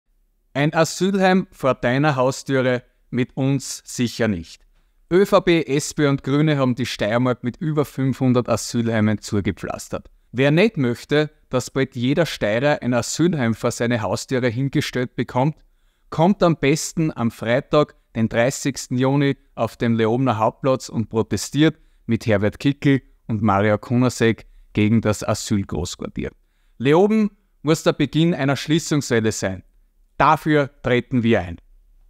LAbg. Marco Triller (FPÖ): Mit Herbert Kickl und Mario Kunasek gegen das Asylquartier in Leoben!
Protestiert am 30. Juni 2023 gemeinsam mit Herbert Kickl und Mario Kunasek gegen das Asyl-Großquartier in Leoben!